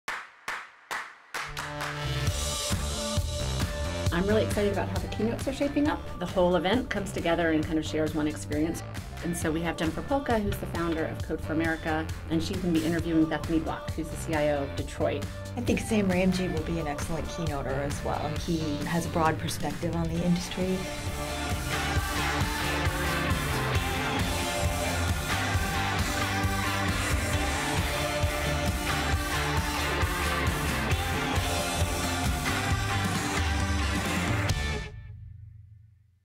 Interop ITX 2018 Keynotes
Hear what Meghan Reilly and Sue Fogarty have to say about this year's Keynote speakers